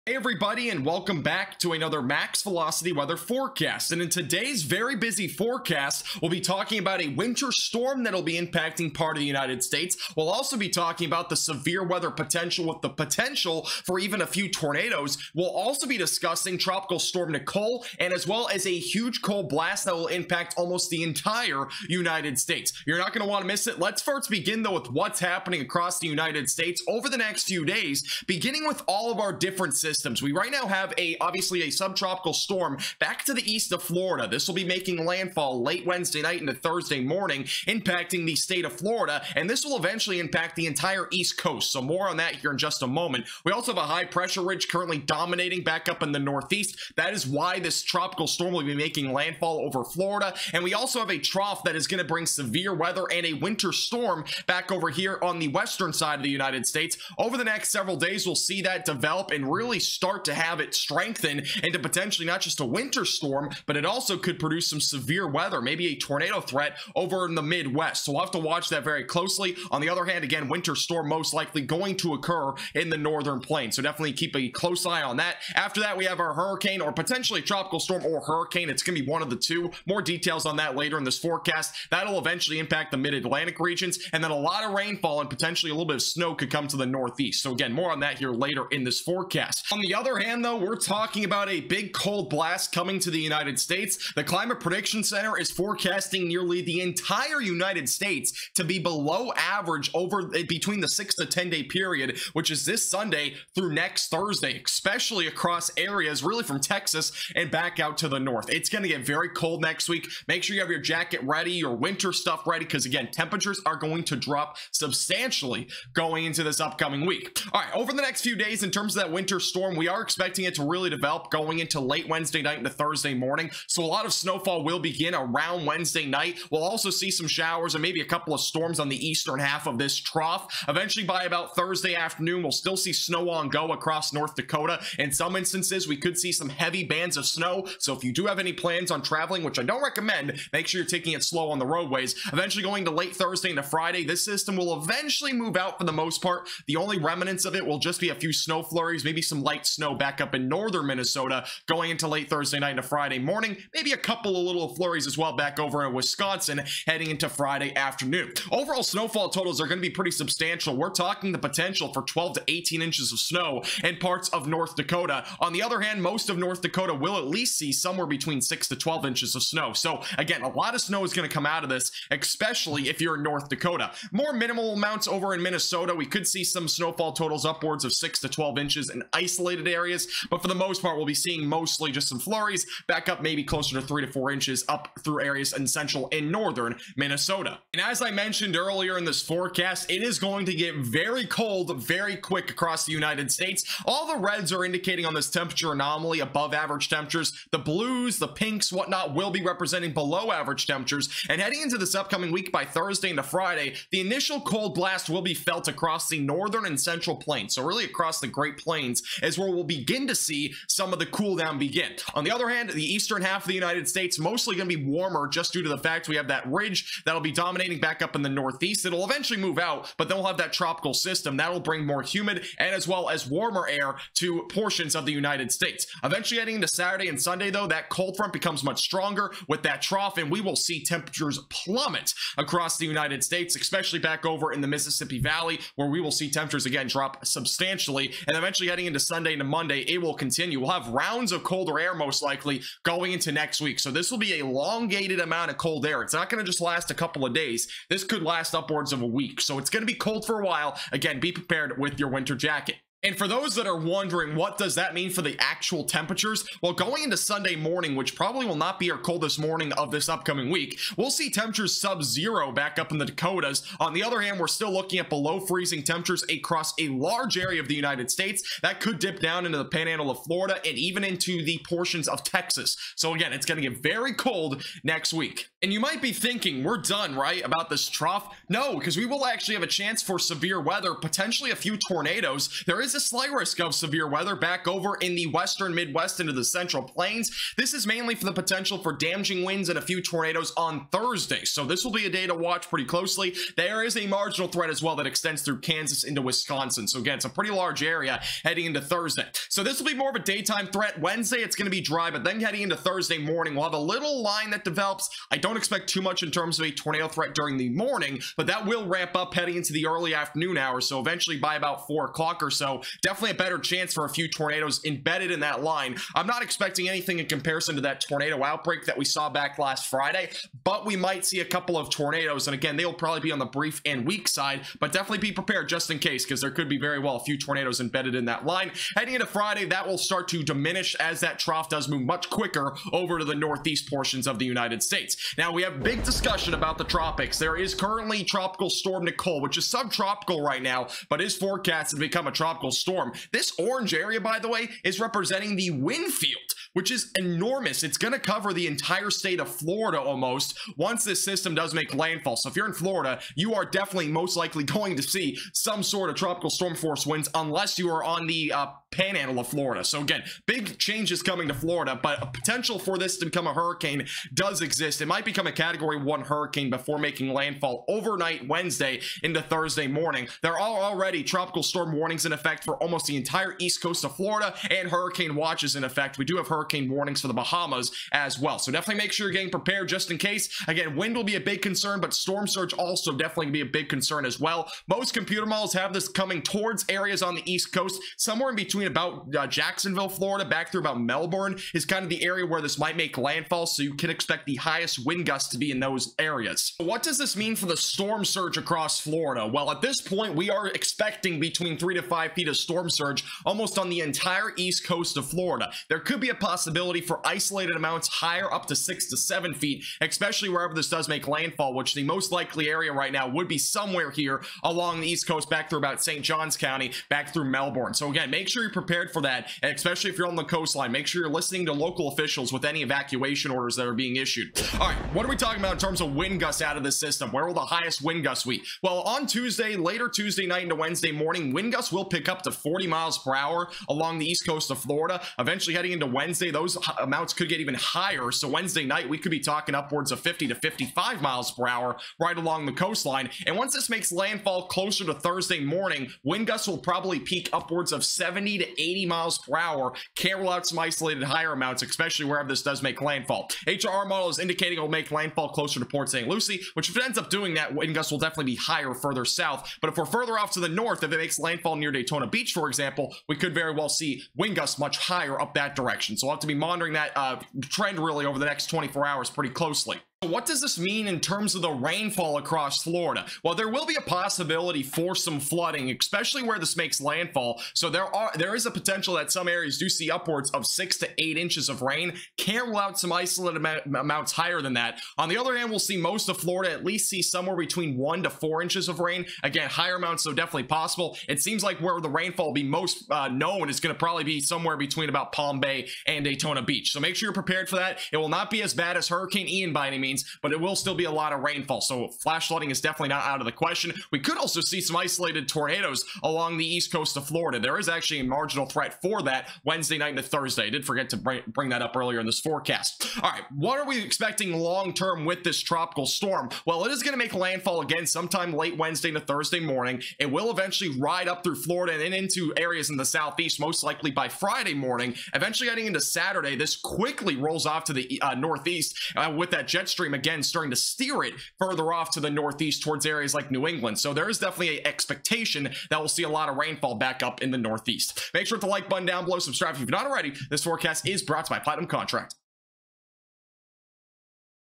Huge Winter Storm To Bring Extreme Snowfall and Tornadoes to USA...
A Winter Storm is coming to the United States which will bring lots of snow, a tornado threat to the Midwest, and a MULTI-DAY Cold Blast that will impact nearly the ENTIRE United States! Also Subtropical Storm Nicole is coming towards Florida and will bring significant storm surge, beach erosion, damaging winds, and more. Tropical Storm Nicole could become a Hurricane as well. Here's everything that YOU need to know!